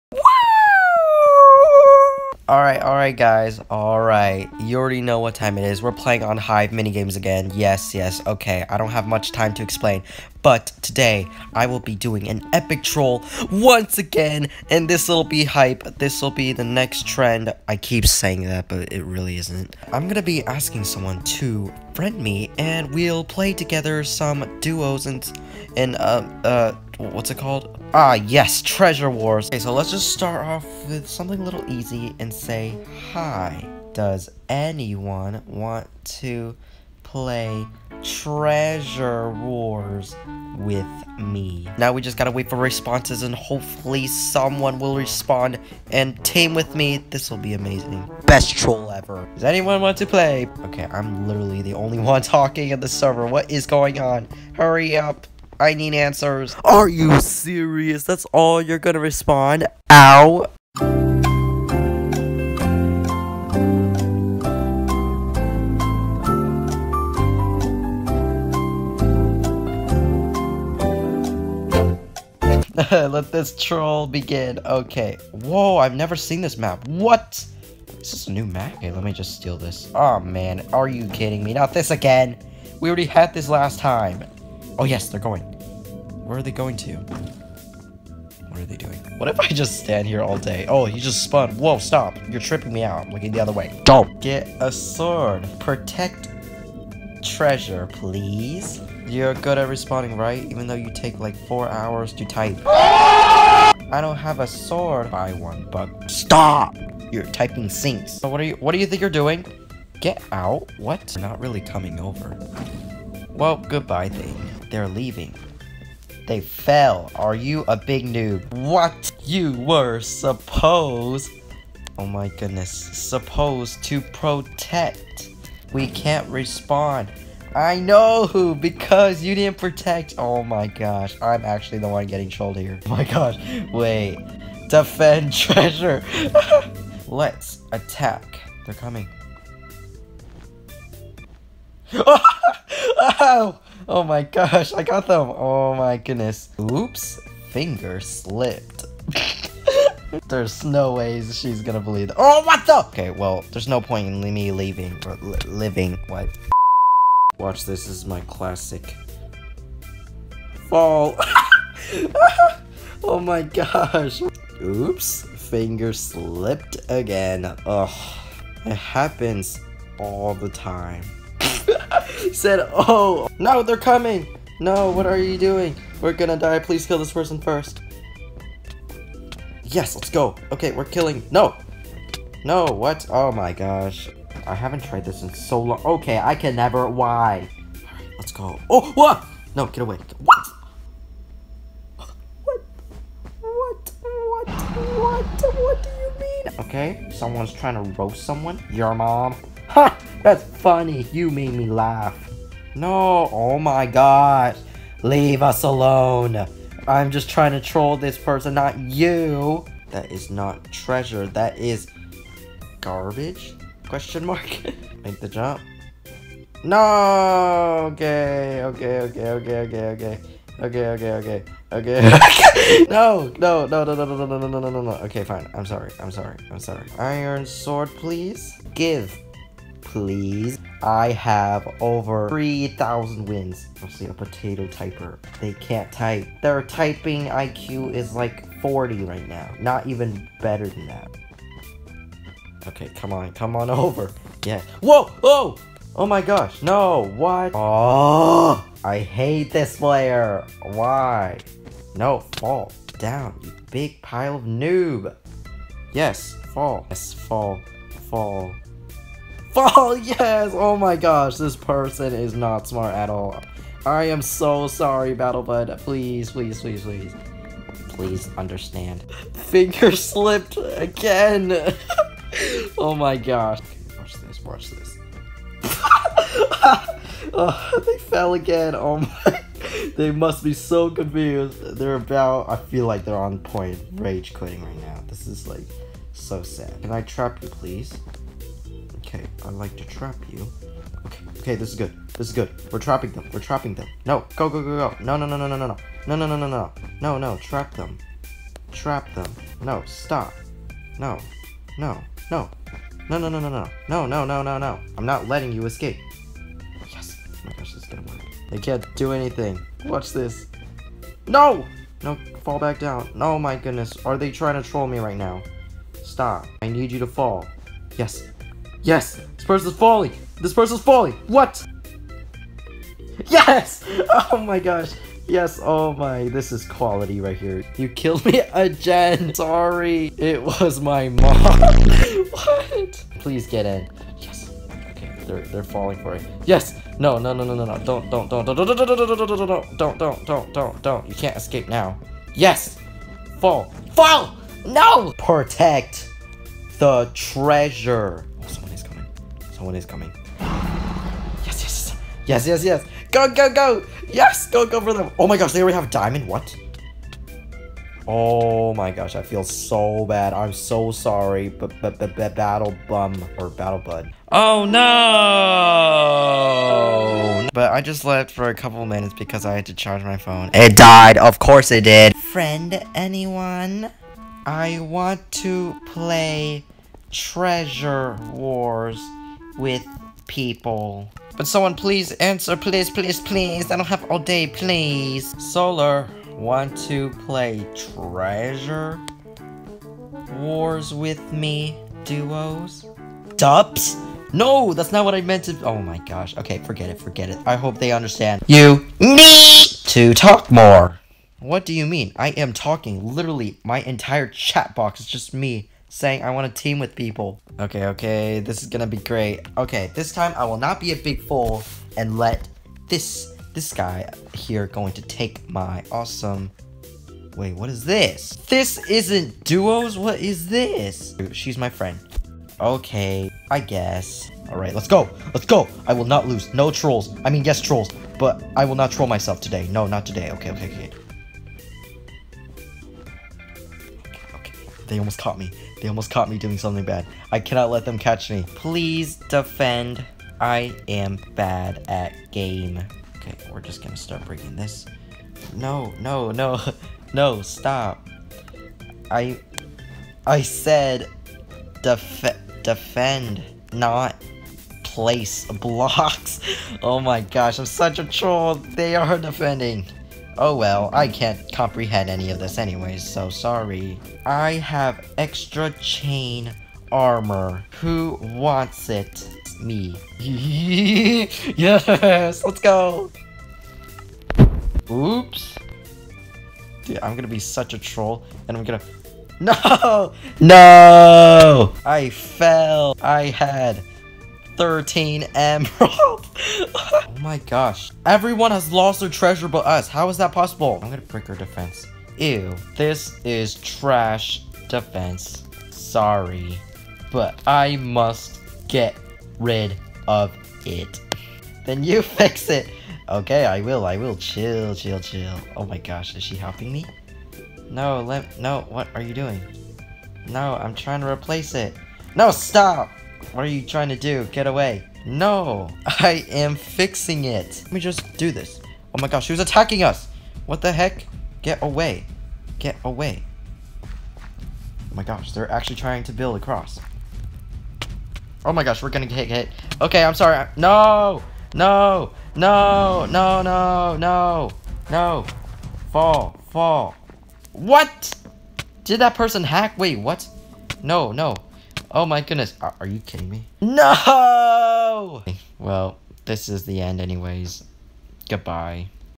trolling my teammate until they RAGE QUIT (confused/angry)
I am still not sure if I am going to be doing a trolling series, I think it’s fun(ny). lie if u saw this thx enjoy 👍